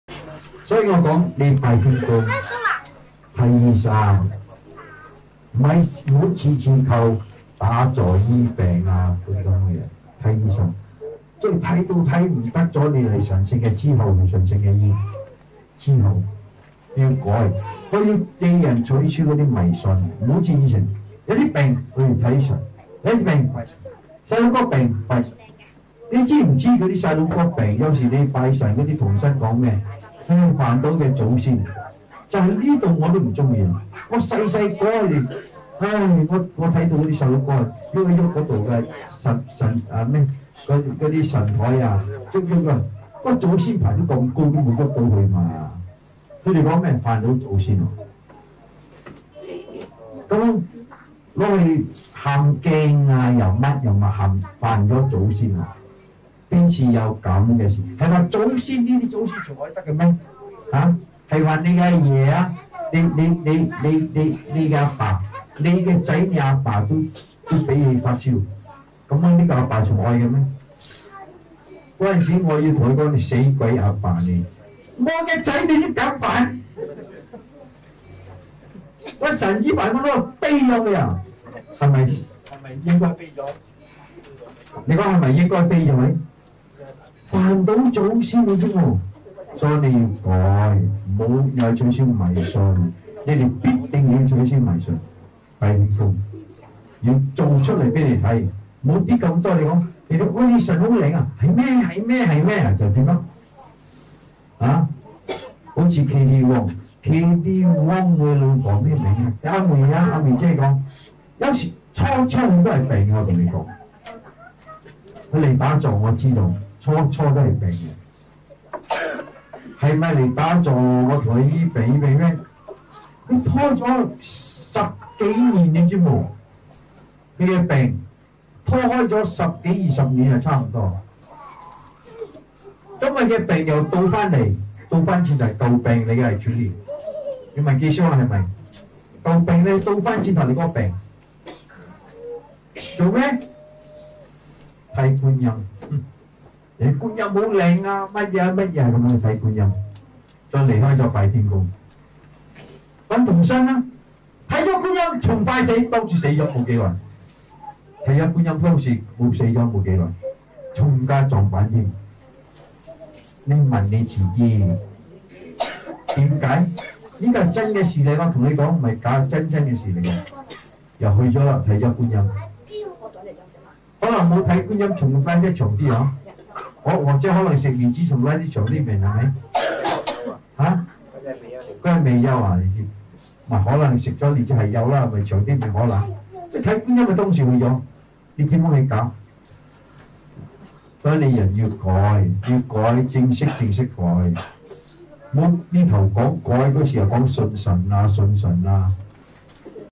16B 2000. (A) Baitiangong followers must get rid of superstitions [01:06 - 04:30]
Xian Sheng has always advocated that followers go to the doctors if they have any illnesses. They should not rely on meditation every time to heal themselves.
If they have exhausted all possible medical treatments, then they may consider seeking spiritual help.
Xian Sheng wanted people to change by eliminating superstition.
In the past, it was common for the Chinese to seek out deities for even the slightest sign of illness, including illnesses affecting their children, in the hope of finding a quick cure.
Xian Sheng asked if the followers are aware that 乩童 (spirit medium) would naturally tell the parents that their children are sick because they offended their ancestors.
Xian Sheng expressed his displeasure with the spirit mediums for deceiving the parents. He recounted an incident when he was very young where he witnessed some children playing near an altar table and accidentally bumping into it. As the ancestral tablet was positioned high up and out of their reach, the children could not have offended it. However, the spirit mediums claimed that the children had offended the ancestors.
The children have to undergo some ritualistic purification for offending their ancestors.
Xian Sheng rubbished their claims and stated that if your departed father is so easily offended by your children and their grandchildren and punishes them by making them sick, then your departed father is good for nothing.
To clarify his argument, Xian Sheng asked if anyone would want to keep the ancestral tablet of their departed father if it made their children feverish.
Xian Sheng stated that if that were to happen to him, he would not only chastise his departed father for daring to harm his children, he would throw his ancestral tablet out of the house.
Xian Sheng asked if the ancestral tablet deserved to be thrown out just for that reason alone - for making their grandchildren sick because of being offended.
Xian Sheng presented this parable to demonstrate that followers must change and eliminate all forms of superstitions.
Baitiangong followers must get rid of their superstitions. They have to show others that they are not superstitious through their actions and behaviour.
If someone were to tell them of an omnipotent deity that can cure all their illness, they should ignore it and not pursue it.
Xian Sheng gave the example of Bro KD Wong's wife. Prior to her joining Baitiangong, she already has a pre-existing illness.
Xian Sheng knew about her pre-existing illness when she came for meditation.
Xian Sheng explained that he helped to ease her of the illness that she had endured for almost twenty years.
However, her illness resurfaced once again.  He asked Bro KC Wong, the brother of KD Wong to verify this.
According to Xian Sheng, the reason was due to her leaving Baitiangong to patronise an omnipotent Kuan Yin deity that someone suggested to her.
However, she died shortly after patronising the Kuan Yin deity.
Xian Sheng reassured the followers that the incident was genuine and not a parable.
Xian Sheng said that she could have lived longer had she not seen the Kuan Yin deity or taken the lingzhi that Bro KC Wong is promoting.
Bro KC Wong replied that he had not yet started promoting lingzhi at that time. Nevertheless, Xian Sheng said that lingzhi could have helped her. But she chose to patronise the Kuan Yin deity which resulted in her untimely demise.
Xian Sheng urged the followers to truly change themselves. The incident with Bro KD Wong's wife should serve as a reminder that followers should not say they changed but immediately turn back to worshipping deities if someone suggested to them that it is omnipotent.